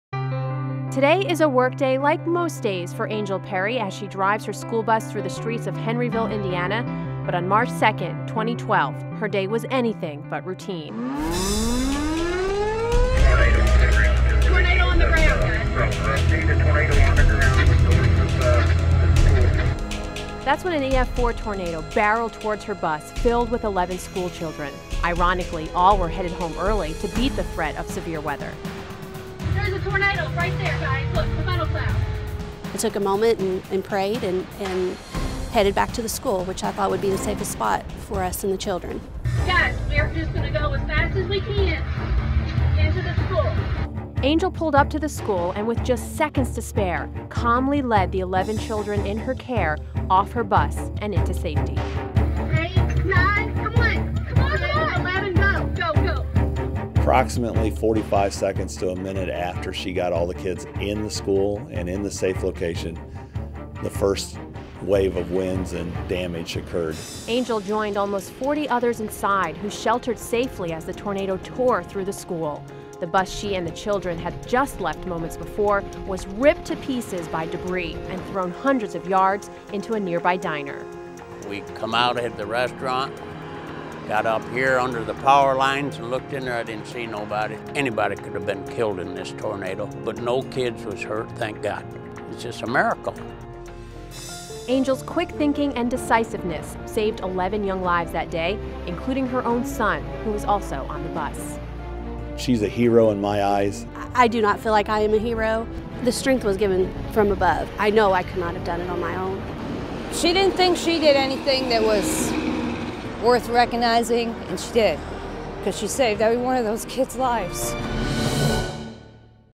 Tornado Hero: Bus Driver Saves Kids
As part of Tornado Week, we are sharing amazing stories of true heroes who risked their lives to save others. This is the incredible story of the school bus driver in Indiana who got her children to safety seconds before a tornado hit.